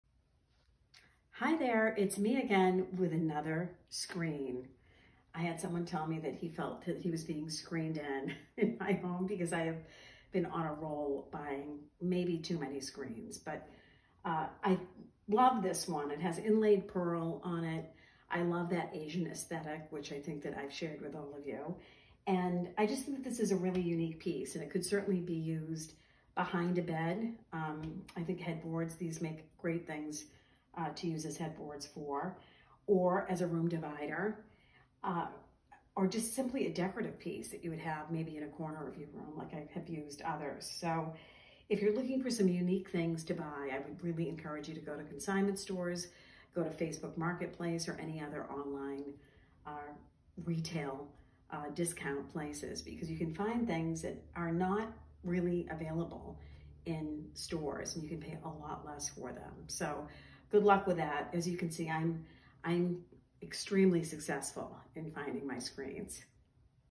Consignment Finds - Room Screen
I LOVE screens...and this is one of my favorites.  Screens are so versatile and can be used in almost any room, I’m not too sure what I’m doing with this one yet, I will have to update you all when I figure it out, but regardless, another great score on Facebook Marketplace!